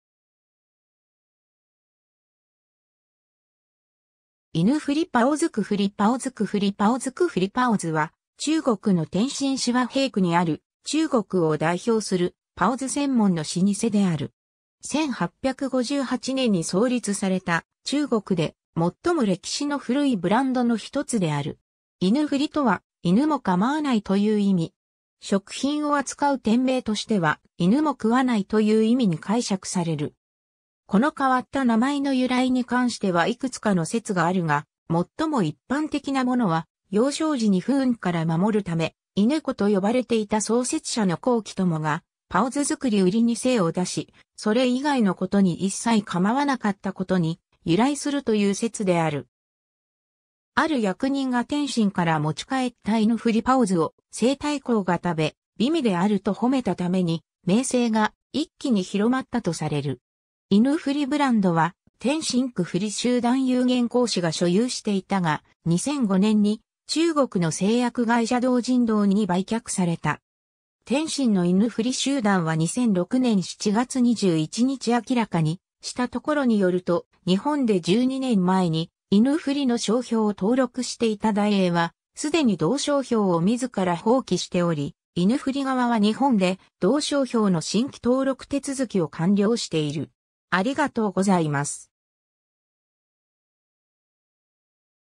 狗不理包子
和平区_(天津市)
狗不理包子 狗不理包子 狗不理包子 狗不理包子（コウプリパオズ、拼音: gǒubùlǐ bāozi）は、中国の天津市和平区にある、中国を代表する包子専門の老舗（中華老字号）である。
 1858年に創立された、中国で最も歴史の古いブランドのひとつである。
 「狗不理」とは「犬もかまわない」という意味。
食品を扱う店名としては「犬も食わない」という意味に解釈される。
この変わった名前の由来に関してはいくつかの説があるが、最も一般的なものは、幼少時に不運から守るため「狗子」と呼ばれていた創設者の高貴友が、包子作り・売りに精を出し、それ以外の事に一切構わなかった（不理）ことに由来するという説である。
 ある役人が天津から持ち帰った狗不理包子を西太后が食べ、美味であると賞めたために、名声が一気に広まったとされる。
 狗不理ブランドは天津狗不理集団有限公司が所有していたが、2005年に中国の製薬会社同仁堂に売却された。
 天津の狗不理集団は2006年07月21日明らかにしたところによると、日本で12年前に「狗不理」の商標を登録していたダイエーは、すでに同商標を自ら放棄しており、狗不理側は日本で同商標の新規登録手続きを完了している。